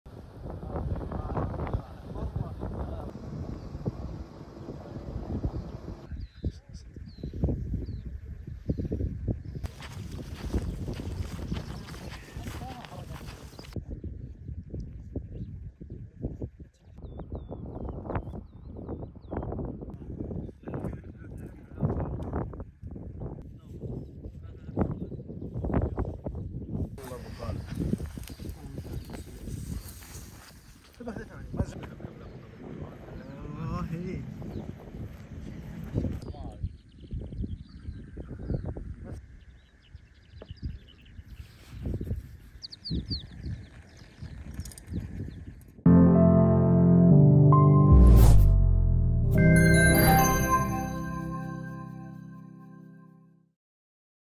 Floral bloom turns Saudi desert purple
A desert bloom triggered by heavier than usual winter rains has carpeted the sands of northern Saudi Arabia with purple flowers, drawing sightseers from across the Arabian Peninsula.